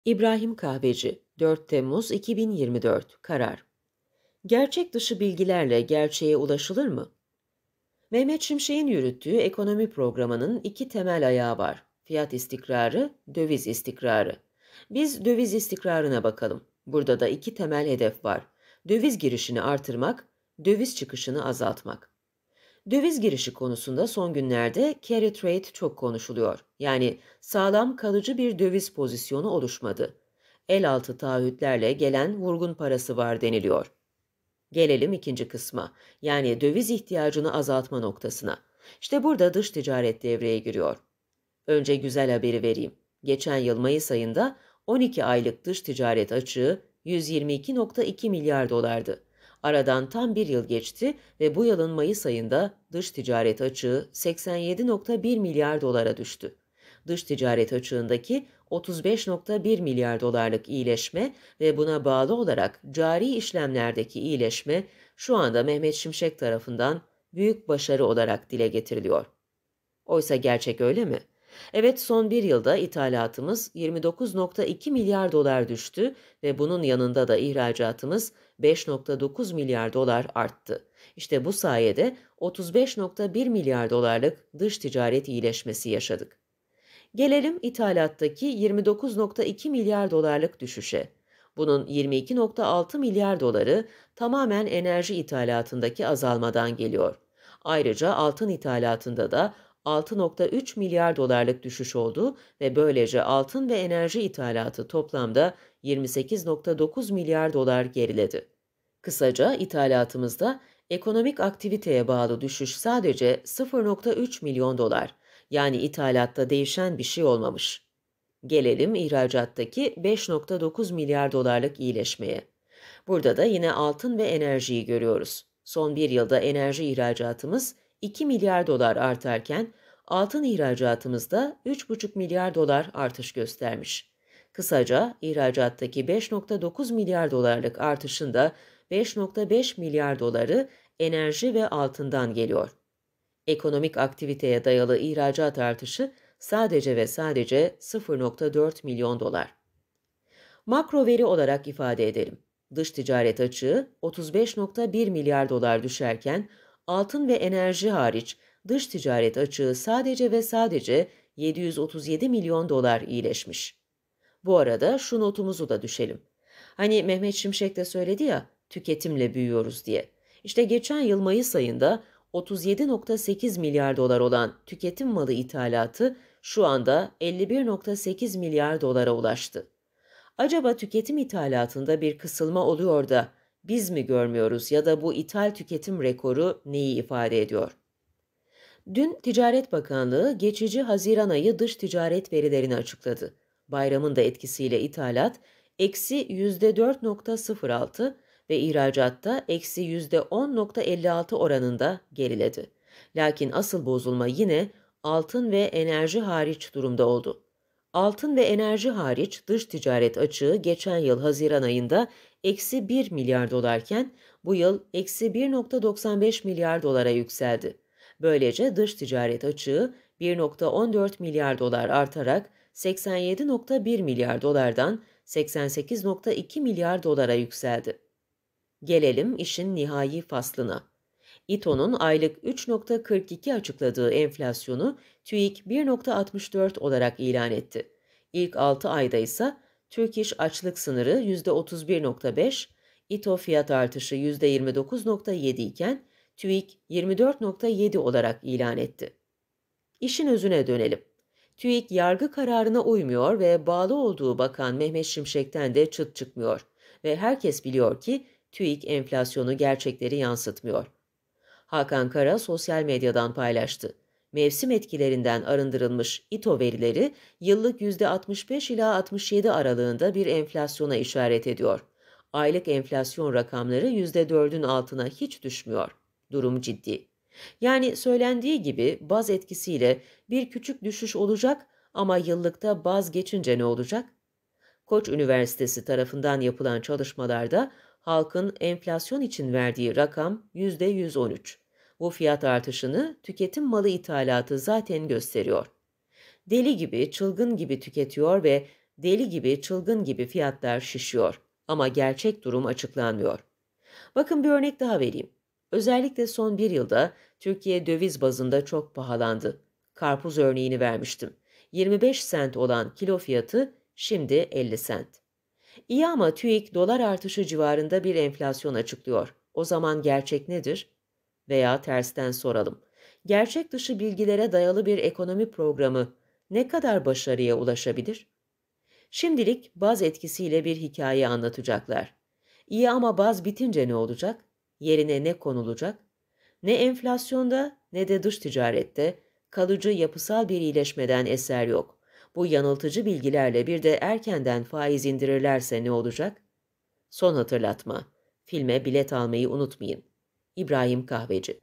İBRAHİM KAHVECİ -"GERÇEK DIŞI BİLGİLERLE GERÇEĞE ULAŞILIR MI?" * Köşe Yazısı Dinle *
Yazar: İBRAHİM KAHVECİ
Tarih : 4 Temmuz 2024
Kaynak : KARAR
Seslendiren: Ersen Ataker Orhun